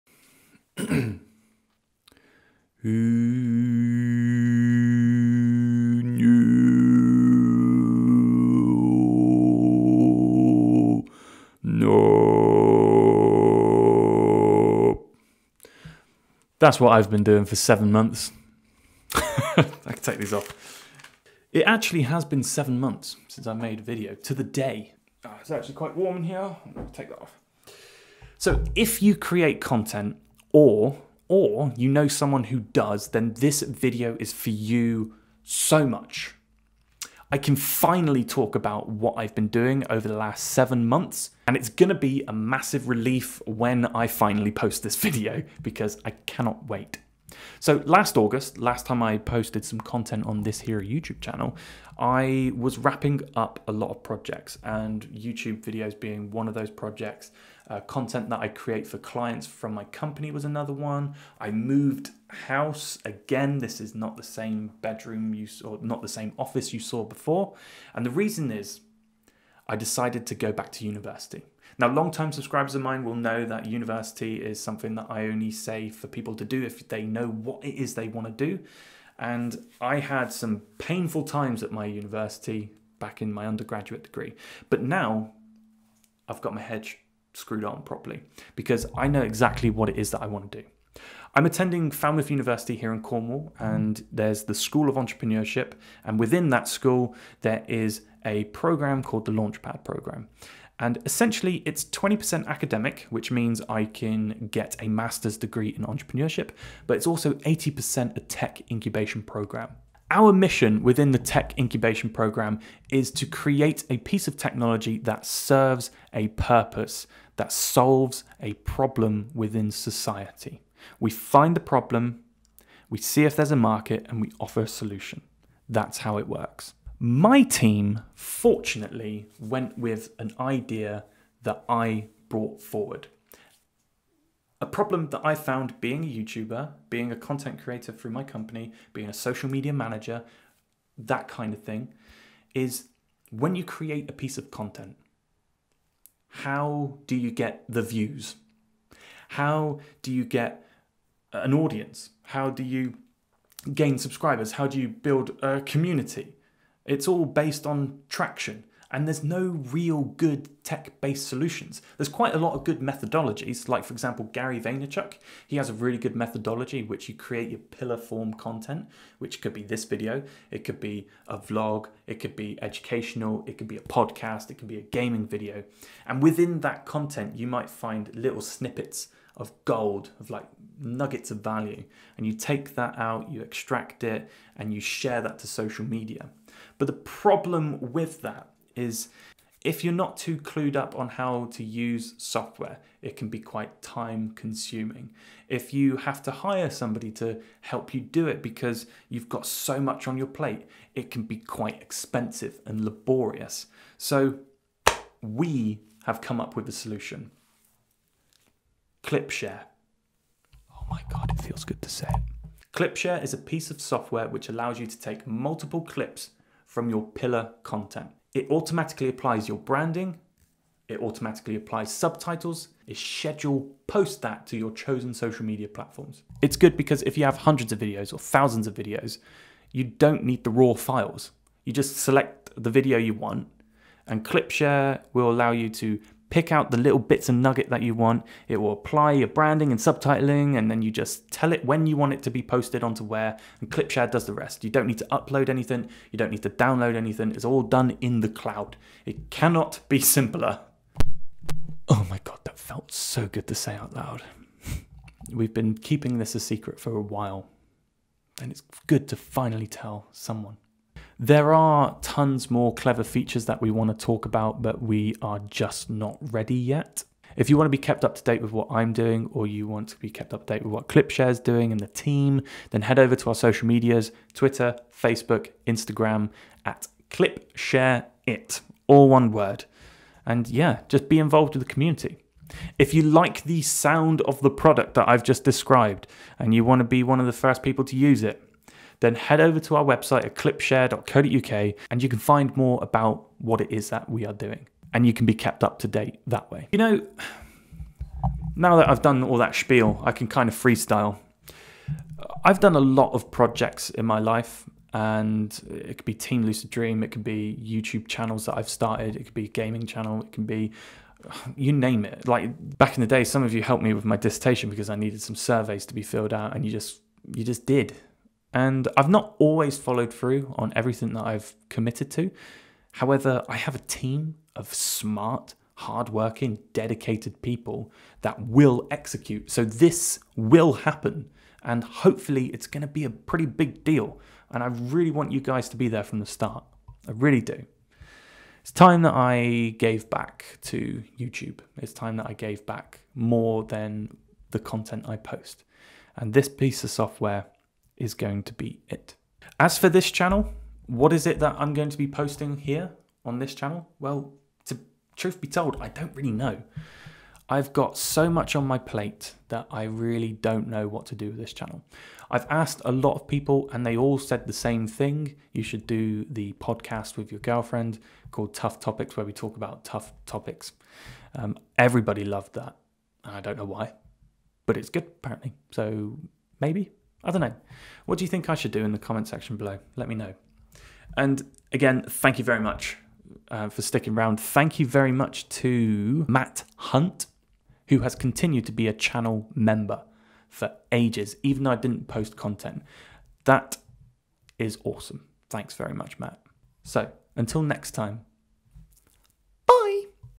I can FINALLY tell you about the last 7 months!!
Unintentional amazing thumbnail.

After seven months of being offline, it is good to finally be able to tell you a little bit about what I have been doing with my time. Believe me it is gonna be a big deal (hopefully)